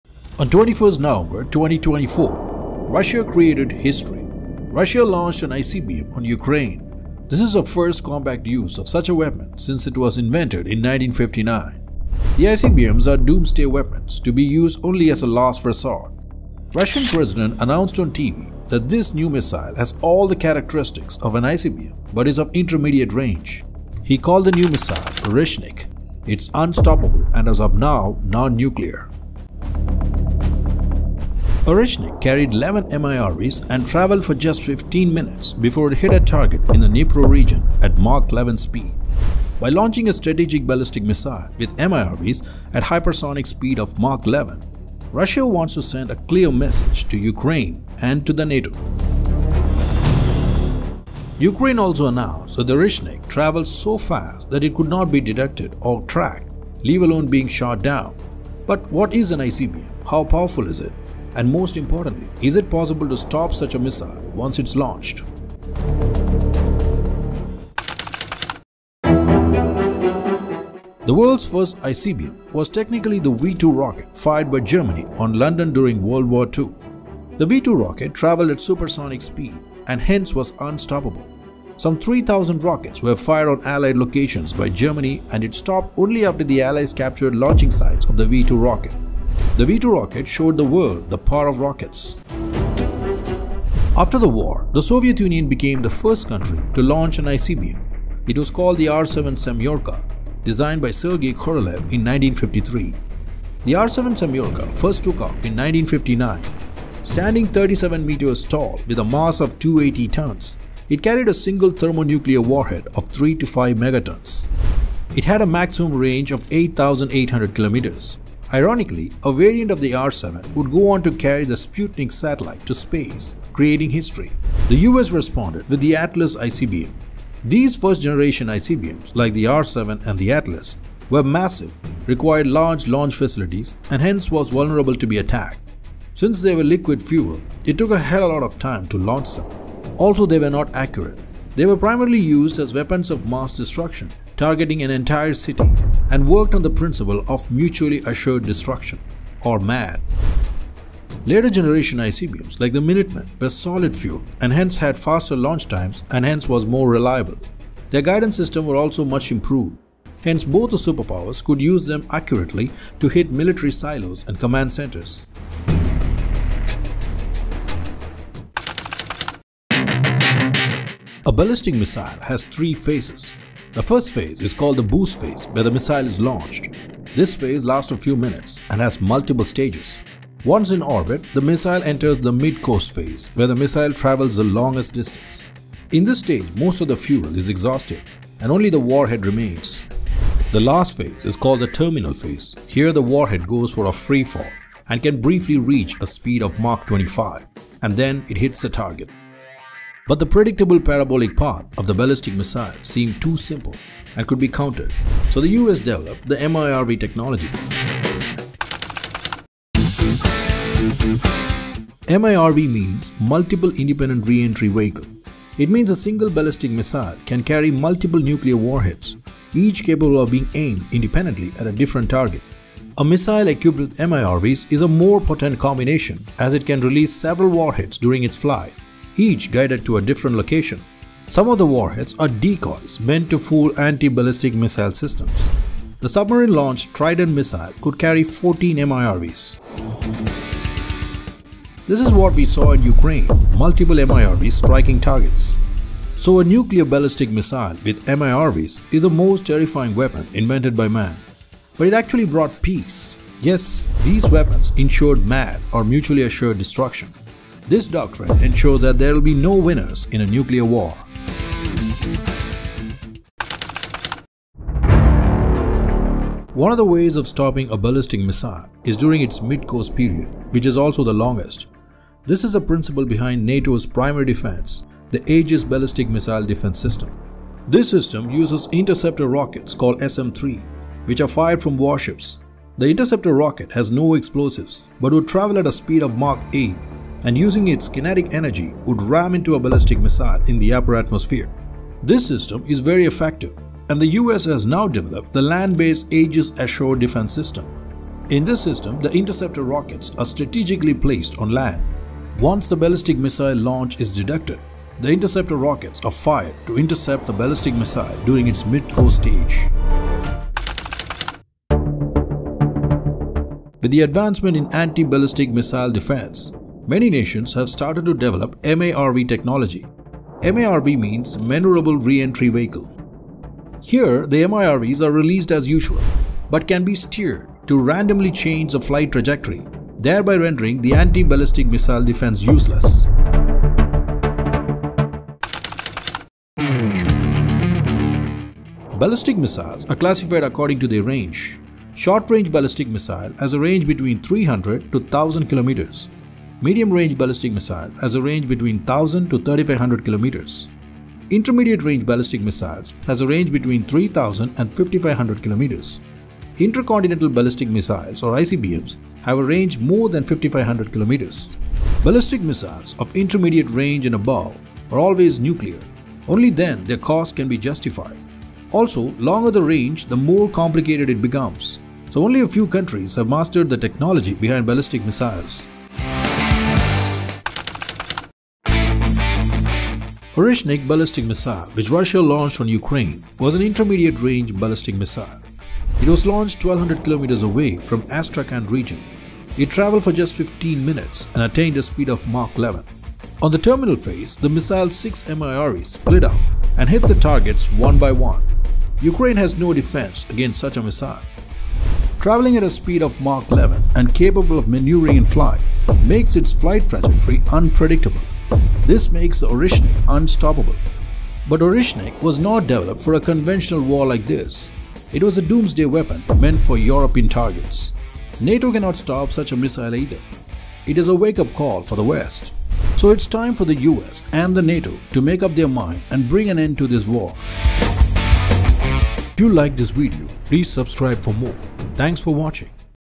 Why "Oreshnik" is Unstoppable ?
Russia launched "Oreshnik",it is unstoppable. Why???

Contents
00:00 - Introduction
01:08 - Cold War
02:48 - How does ICBM Work?
03:28 - MIRV
04:23 -Ballistic missile technology
05:10 -MaRV
05:35 -Ballistic Missile Types
06:17 -Oreshnik